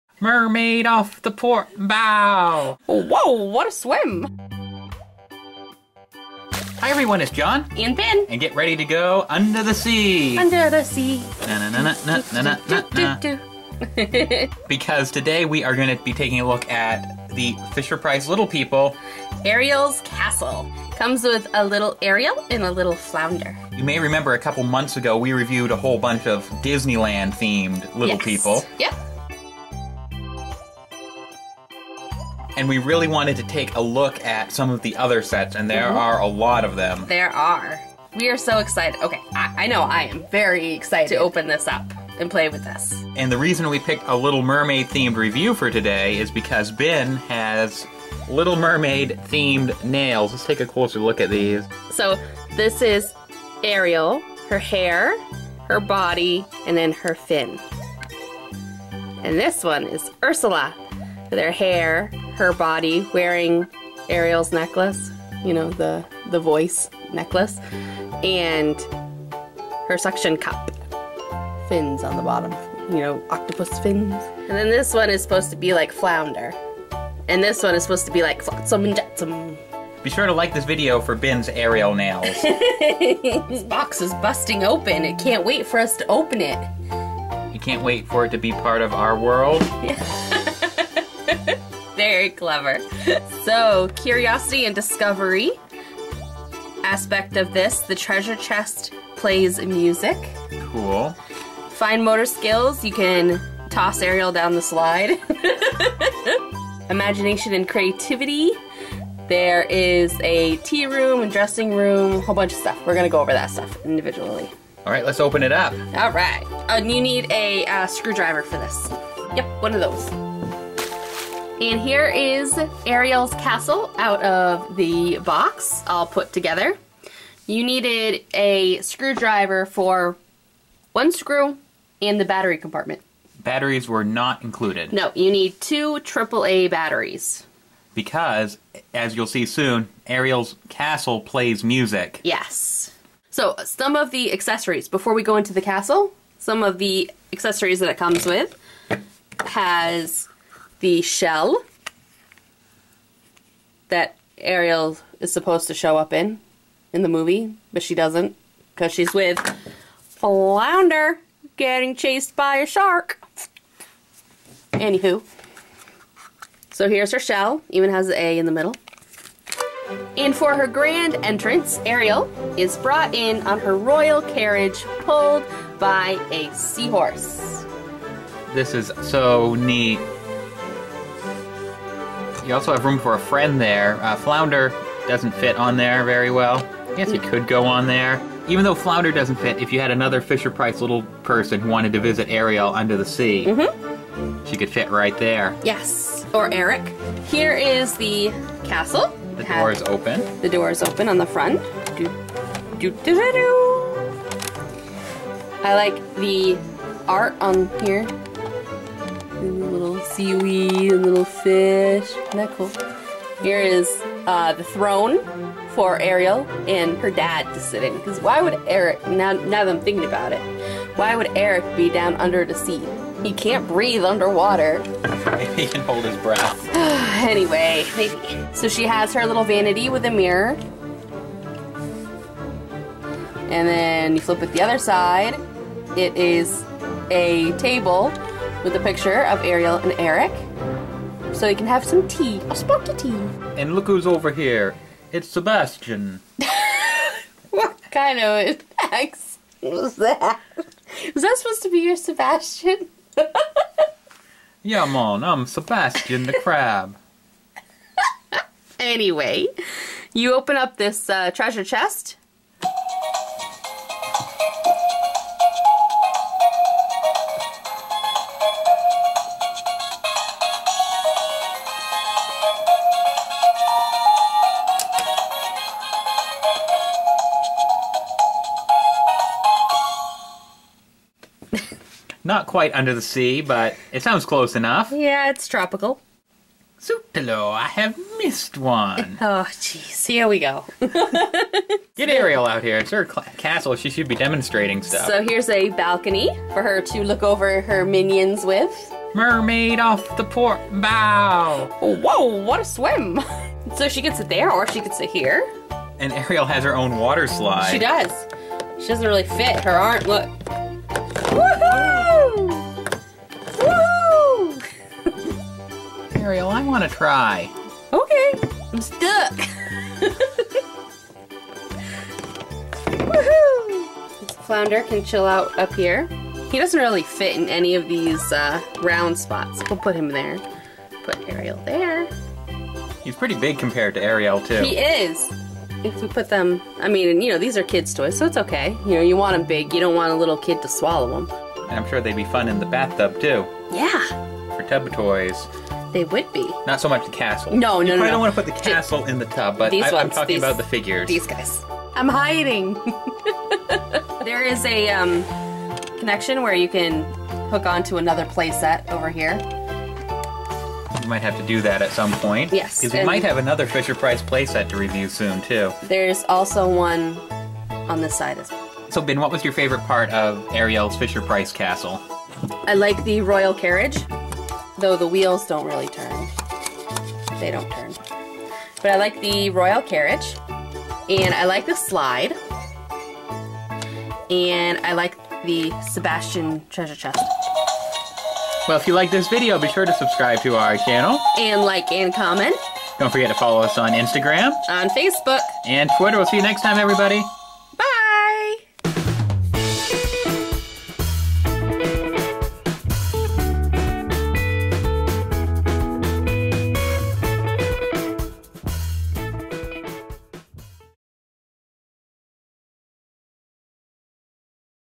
Disney Little Mermaid Ariel's Castle Fisher-Price Little People Playset! Review by Bin's Toy Bin
Time to go under the sea with this fun toddler toy from Fisher-Price Little People! Disney's Little Mermaid- Ariel's Castle!  Comes with figures of Ariel and Flounder, plus lots of fun play features including a musical treasure chest, a slide, a seahorse-drawn carriage, and more!

BinsToyBin.com

Bin's Toy Bin logo created by Anna Taylor.